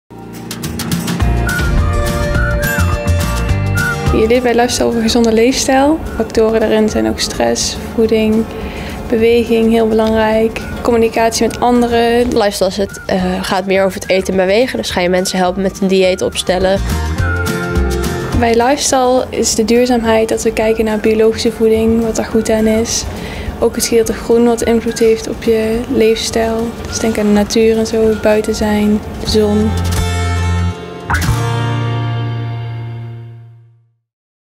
Wil je iets met Stad en mens?........Lifestyle
Wil jij mensen helpen met een gezond eetpatroon, goede werkhouding en het verminderen van stress? Dan past Lifestyle bij jou! Je leert wat een gezonde levensstijl is en welke rol natuur, voeding en beweging hierbij spelen. Met je mbo-diploma kun je vele kanten op. Doorstuderen op het hbo of aan het werk in de gezondheidssector.